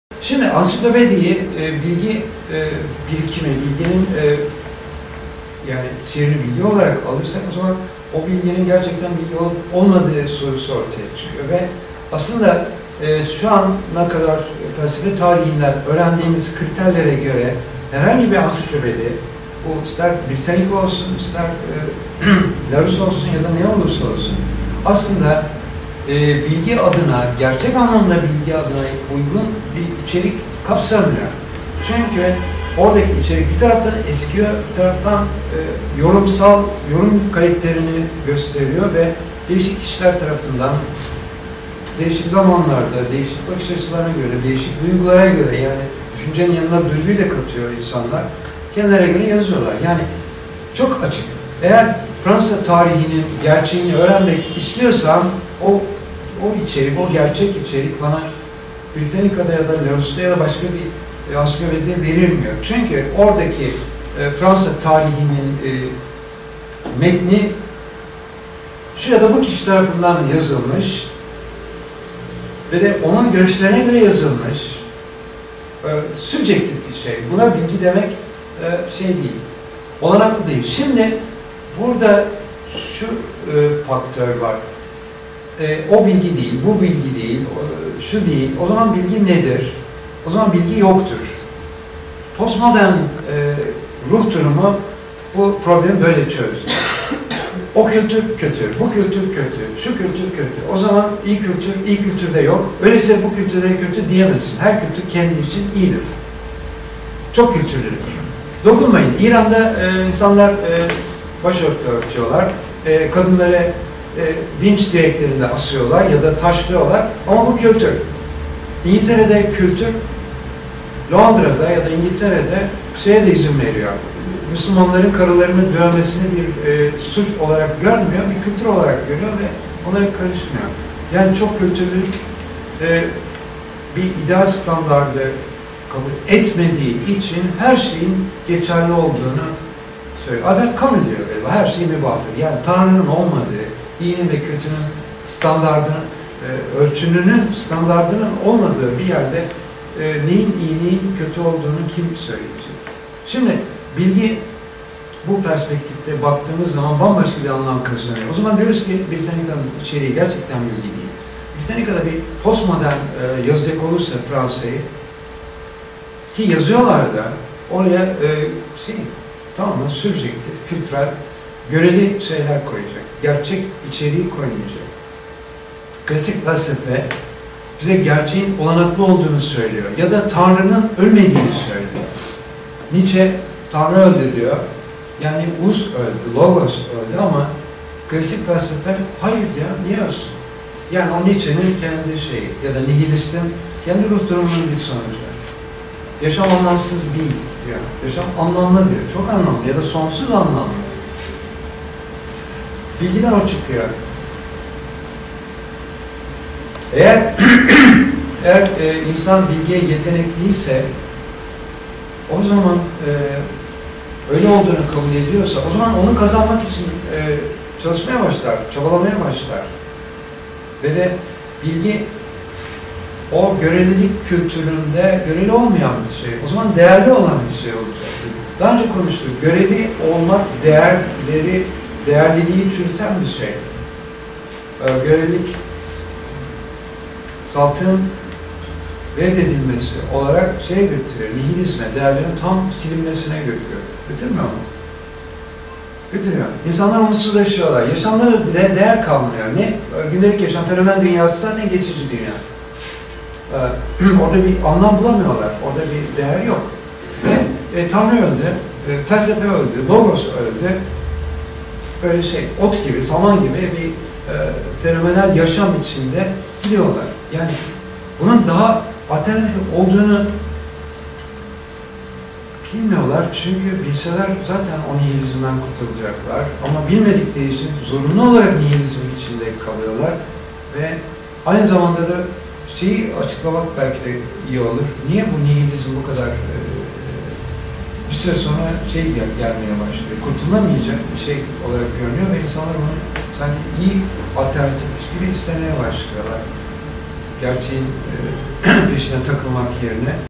Görelilikten Nihilizme
Bu fragman 2010_11_29 tarihinde Rönesans Sanatevi'nde yapılan Mantık Bilimi çalışmasından alındı.